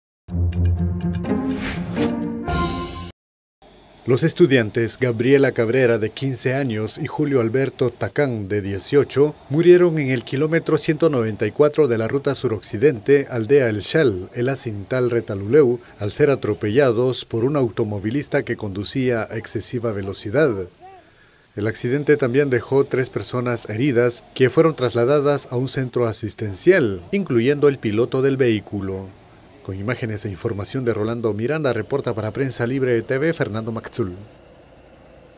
Dos estudiantes mueren atropellados en Retalhuleu
Los estudiantes mueren atropellados en el km 194 de la ruta a suroccidente, aldea El Xal,  El Asintal, Retalhuleu. (Video Prensa Libre: R. Miranda y F. Magzul).